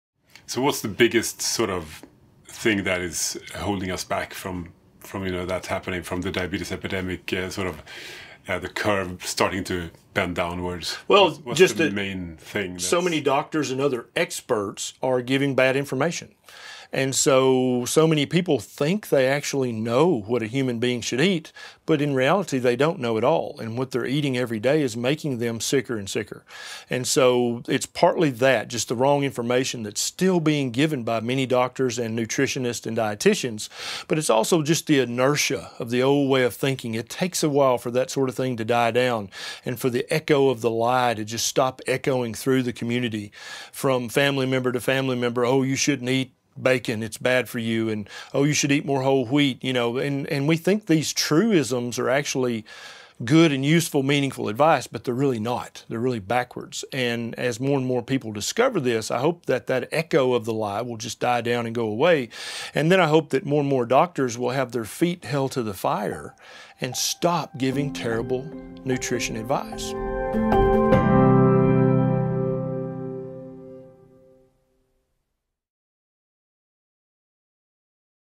[Preview] "I was becoming that stereotypical fat doctor" — Dr. Ken Berry
Dr. Ken Berry, MD, is a practicing family doctor located in the state of Tennesse. In this interview, Dr. Andreas Eenfeldt sits down to discuss what made Ken turn to a keto diet and what it’s been like treating his patients with the diet. With the amazing results on his own health, as well as his patients, he wanted to spread the message, so he started his own YouTube channel.